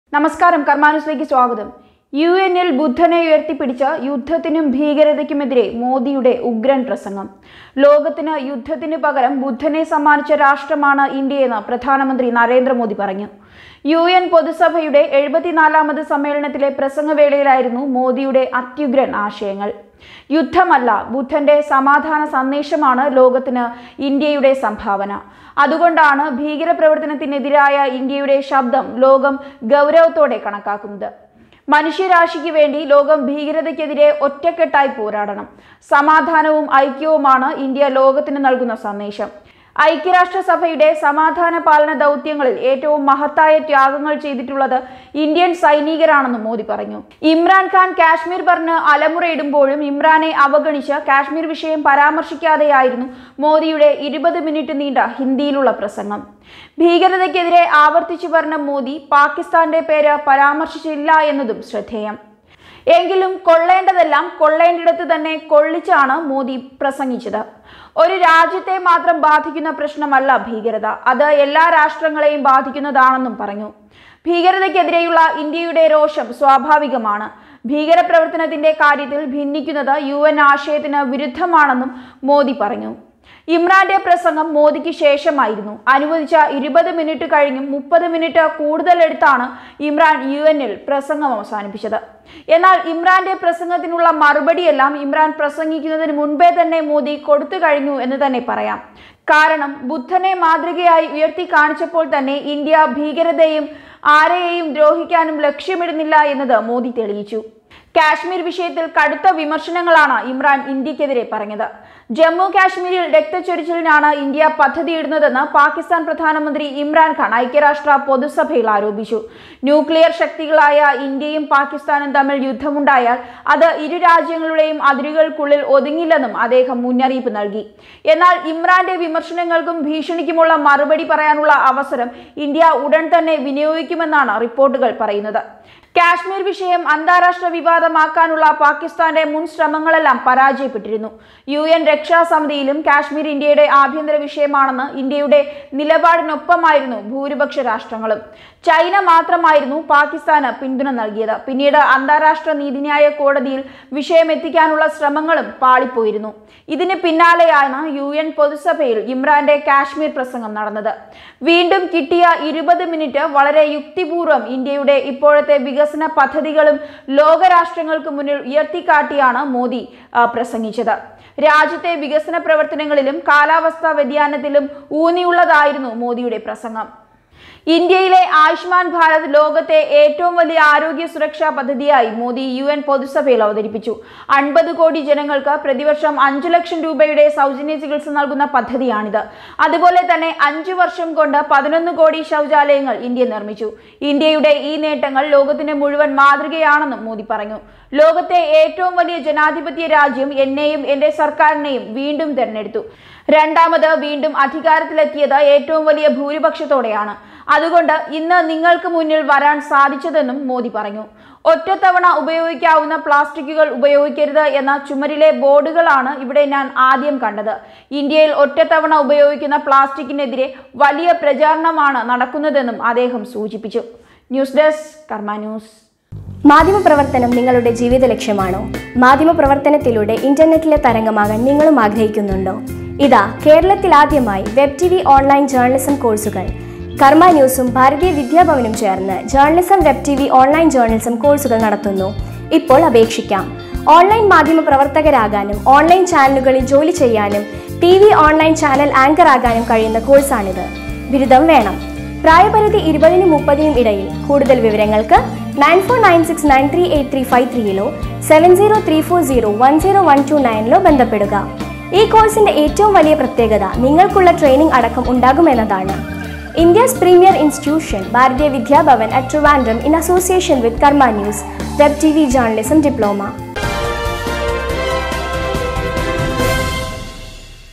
യു എന്നിൽ ബുദ്ധനെ ഉയർത്തിപ്പിടിച്ച് ഇമ്രാനിട്ട് കൊട്ട് കൊടുത്ത് മോദിയുടെ ഉഗ്രൻ പ്രസംഗം | karma news
ലോകത്തിന് യുദ്ധത്തിന് പകരം ബുദ്ധനെ സമ്മാനിച്ച രാഷ്‌ട്രമാണ് ഇന്ത്യ;ഭീകരതയുടെ വേരറുക്കുന്നതു വരെ  ഇന്ത്യ പോരാടും .ഭീകരന്മാരെ വച്ചുപൊറുപ്പിക്കില്ലെന്നും ; ഇമ്രാന്റെ മുന്നിൽ നിന്ന് പാക്കിനെ  തൊട്ടു മോദിയുടെ കിടിലൻ പ്രസംഗം